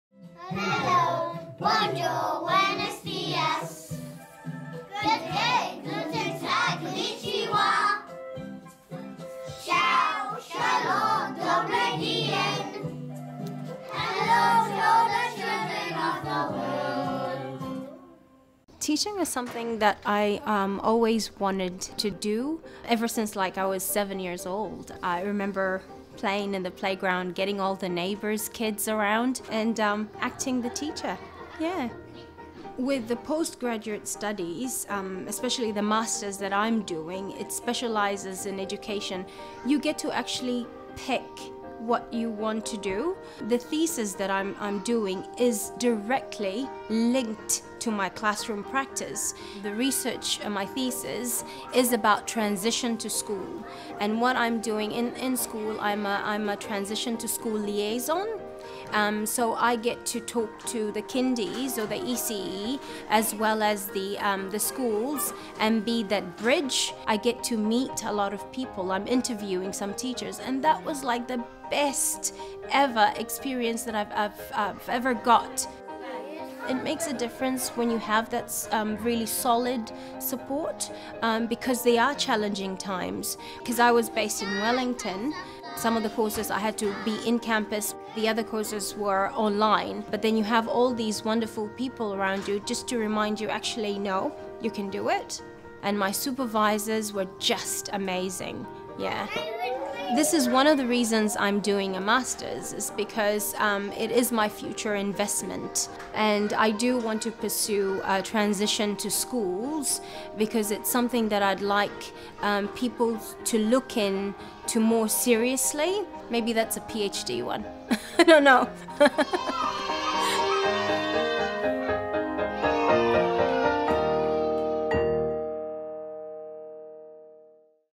Why I'm doing a masters degree in Education at the University of Auckland
Fatma is making her masters study meaningful for her. Her taught courses and research is directly linked to her teaching practice and focused around children's transition to school.
You can find out more about postgraduate study with the Faculty of Education and Social Work